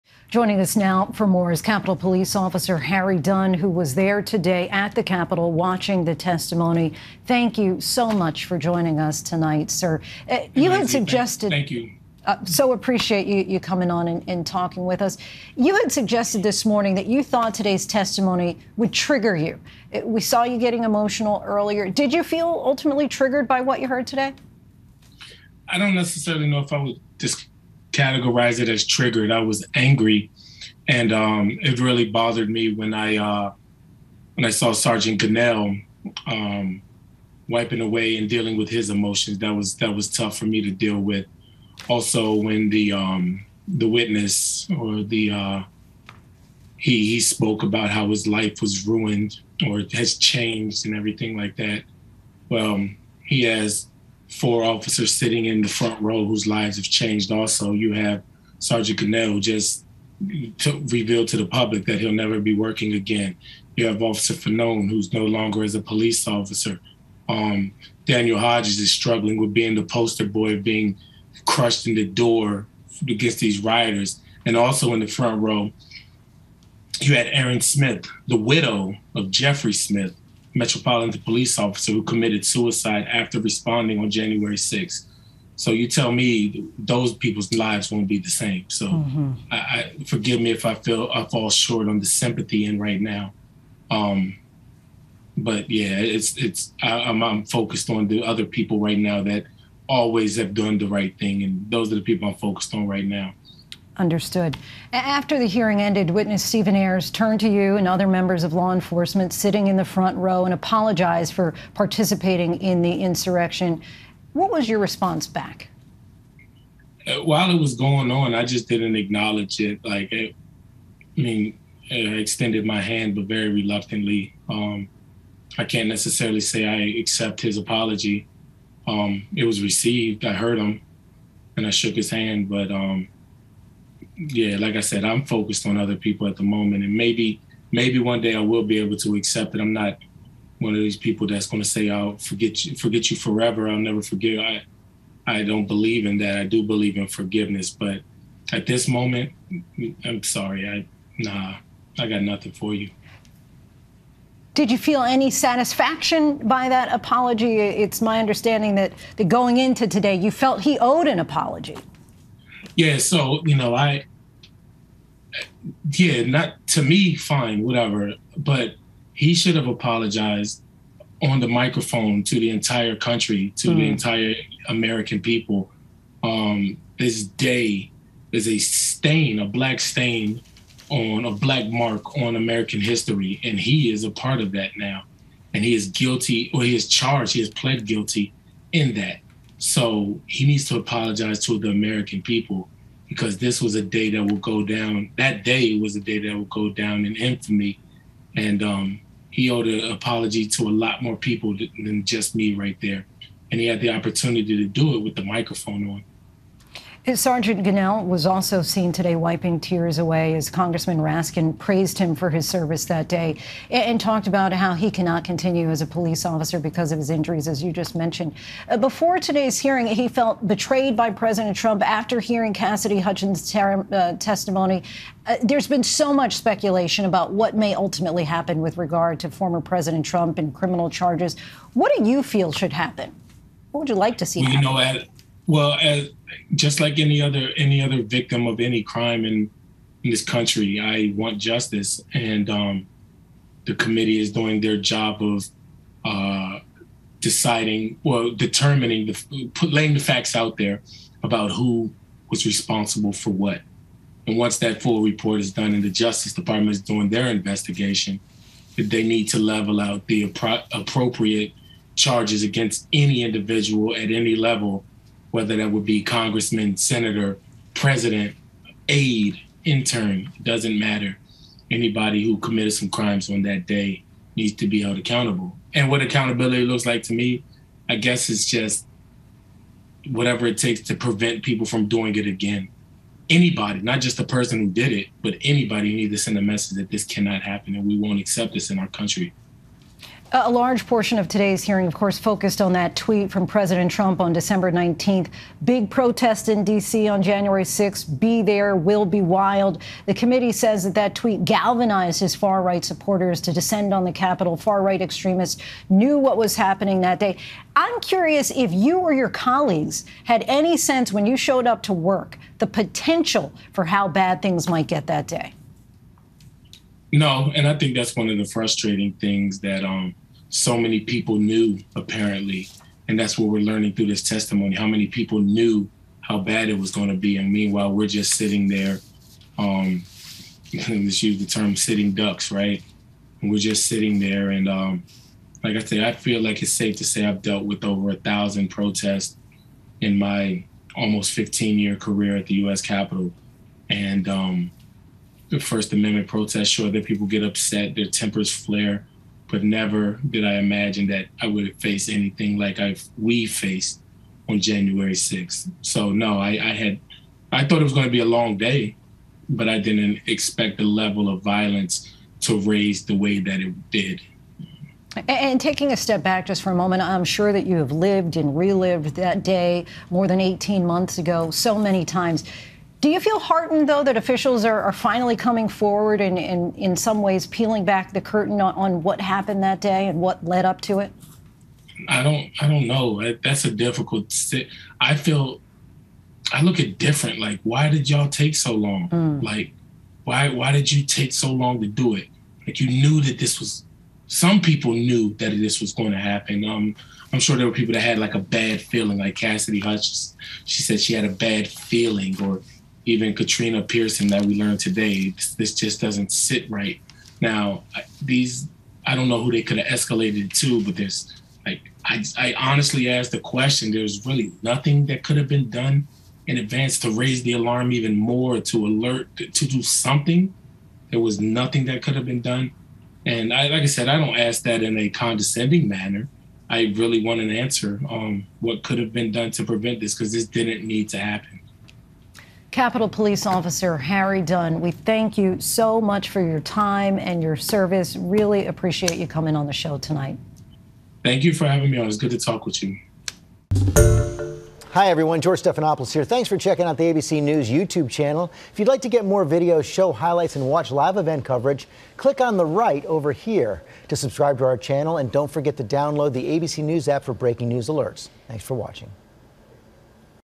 Capitol Police officer on testimony: ‘Forgive me if I fall short on sympathy'
ABC News’ Linsey Davis spoke with Capitol Police Officer Harry Dunn about his experience at the Jan. 6 hearings and what he feels should happen to Former President Donald Trump now.